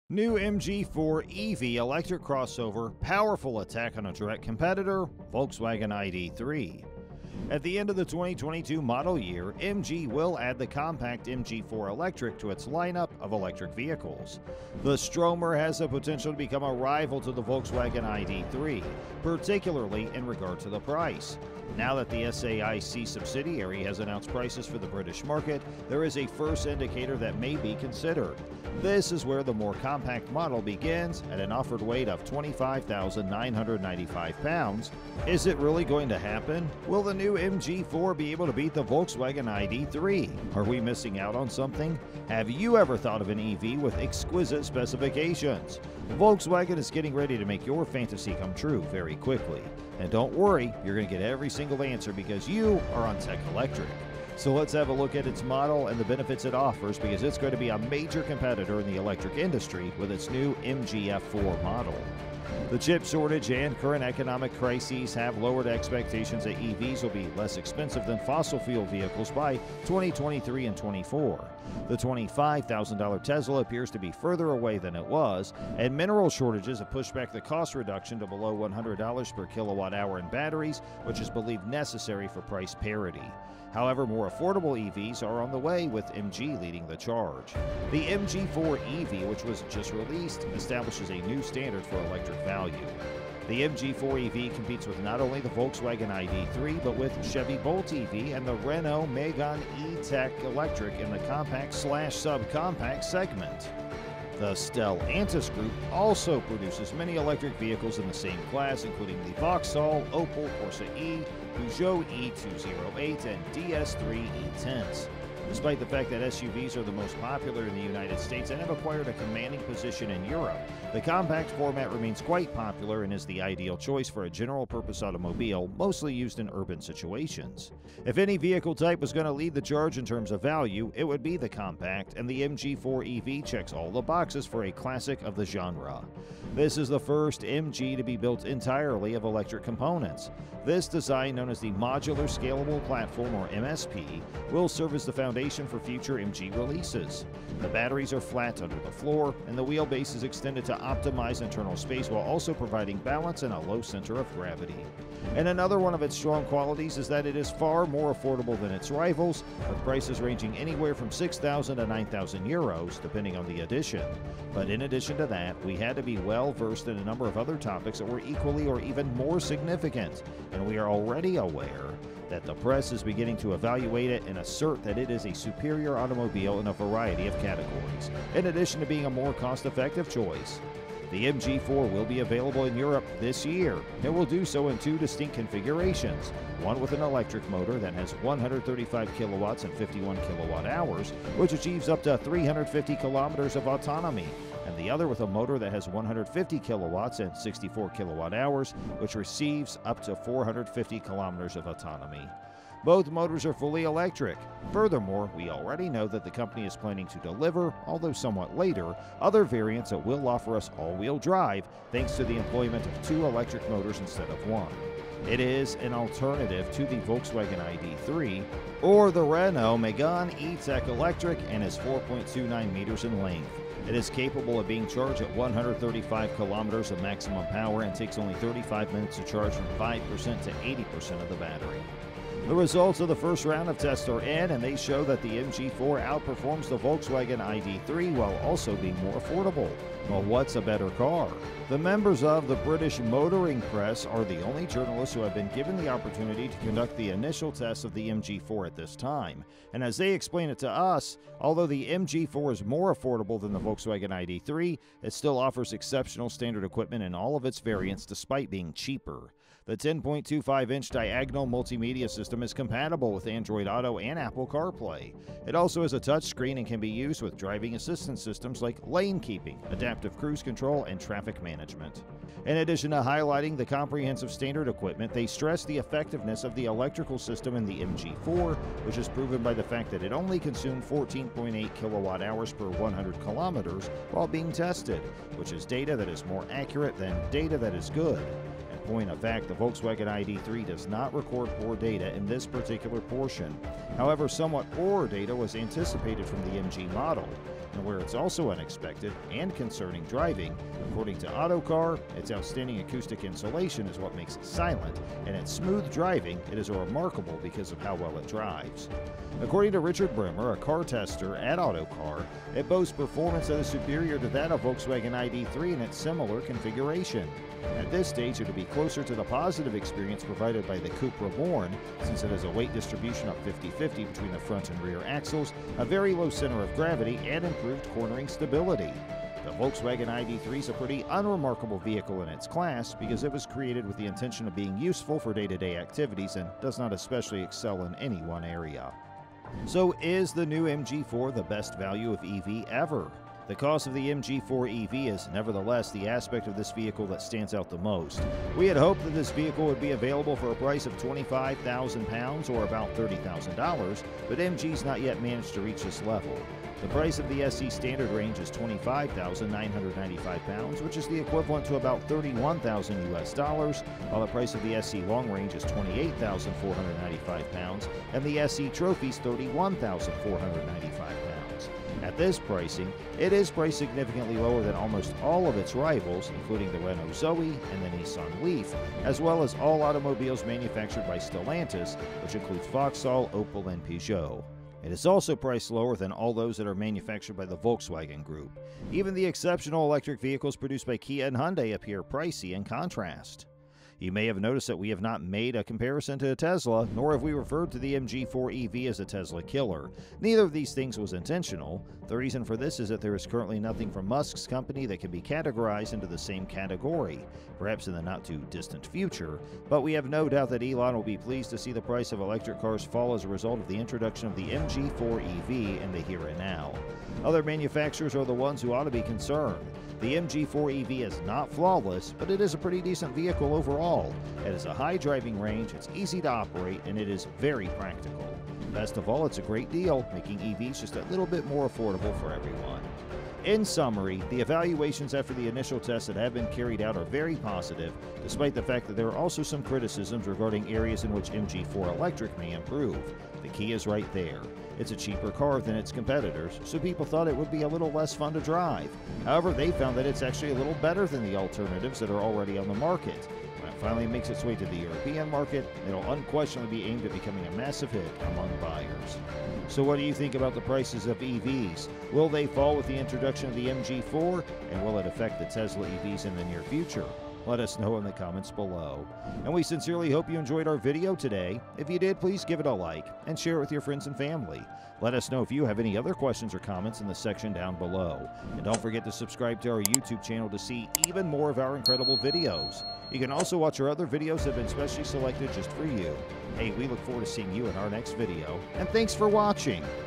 HORROR!! MG4 Powerful ATTACK Volkswagen ID 3
At the end of the 2022 model year, MG will add the compact MG4 Electric to its lineup of electric vehicles. The Stromer has the potential to become a rival to the Volkswagen ID.3. Particularly in regard to the price. Now that the SAIC subsidiary has announced prices for the British market, there is a first indicator that may be considered. This is where the more compact model begins at an offered weight of 25.995 pounds, which at the moment amounts to around CHF 30.200 is equivalent to.

Is it really going to happen?  Will the new MG4 be able to beat the Volkswagen ID 3? Are we missing out on something? Have you ever thought of an EV with exquisite specifications? Volkswagen is getting ready to make your fantasy come true very quickly. Don't worry you will get every single answer because you are on TECH ELECTRIC. So,  Let's have a look at its model and the benefits it offers because it is going to be a major competitor in the electric industry with its new MGF4 model.

The chip shortage and current economic crises have lowered expectations that EVs will be less expensive than fossil-fuel vehicles by 2023/24. The $25,000 Tesla appears to be further away than it was, and mineral shortages have pushed back the cost reduction to below $100 per kWh in batteries, which is believed necessary for price parity. However, more affordable EVs are on the way, with MG leading the charge. The MG4 EV, which was just released, establishes a new standard for electric value.

About Tech Electric
🎥 Videos about Electric Vehicles (EV), EV Batteries, Nuclear Diamond Battery and more
🎨 Written, voiced and produced by Tech Electric